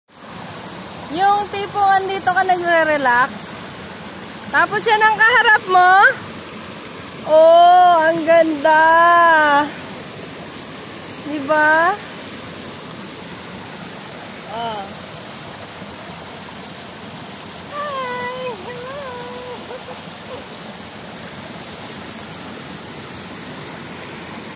Nature spring in our Lemon Farm
When you love nature spring and it is just in your farm side. You will love it more and more and it is so relaxing.